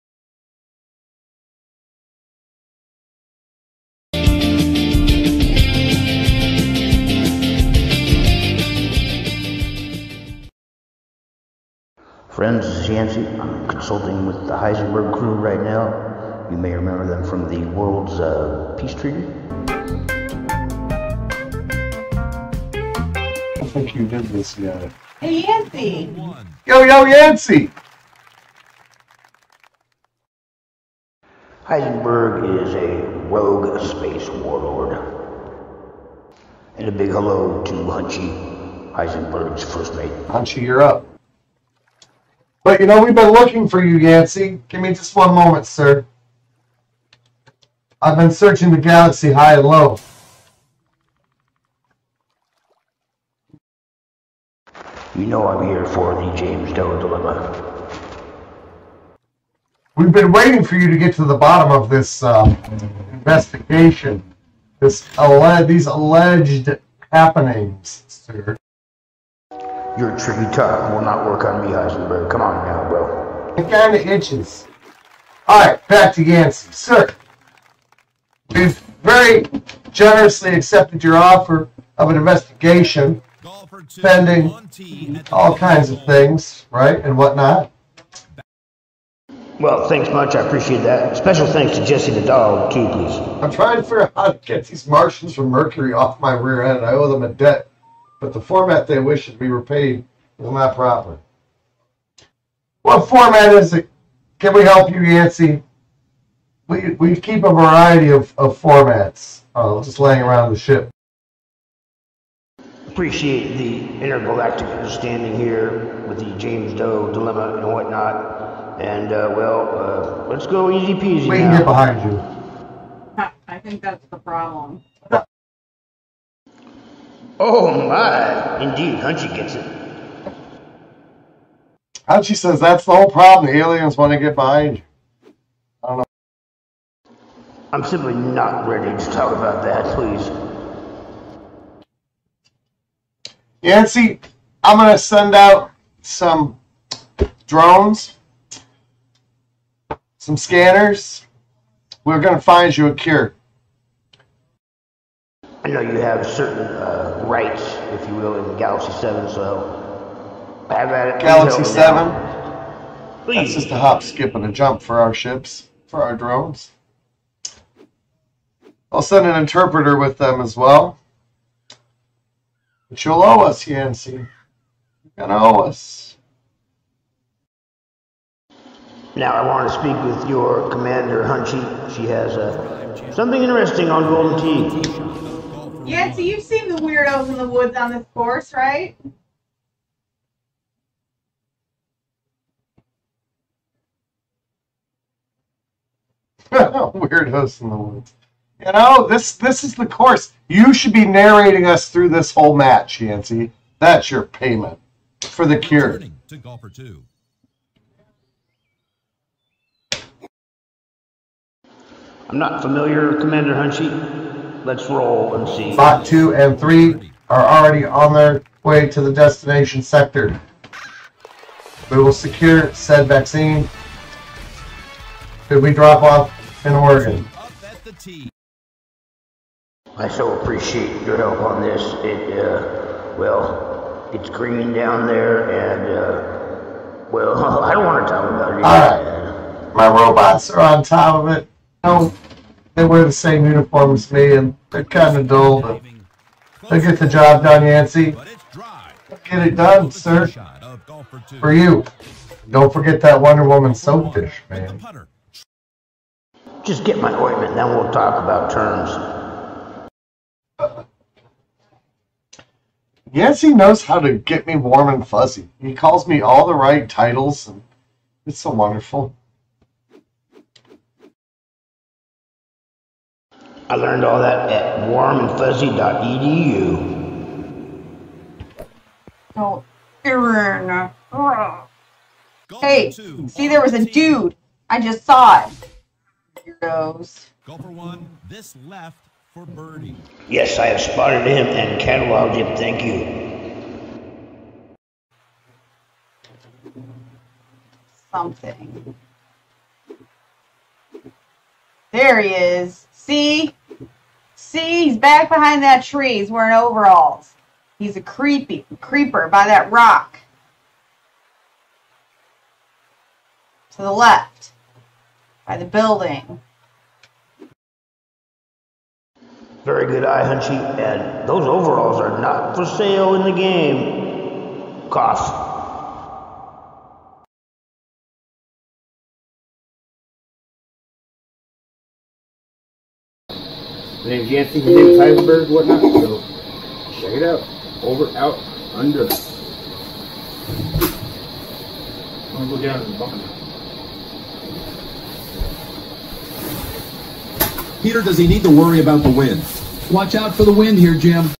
Team Yancy & Team Heisenberg Intergalactic Roundtable #6 - OIntments, James Doe, Creepers & Overalls
Team Yancy & Team Heisenberg - seen here in this RAW footage - discuss the issues we face daily in GoldenTee Golf. This time we touch upon: Galaxy7, Creepers in Overalls, James Doe & much more.
Any Questions - Throw Them Here!
Best, I am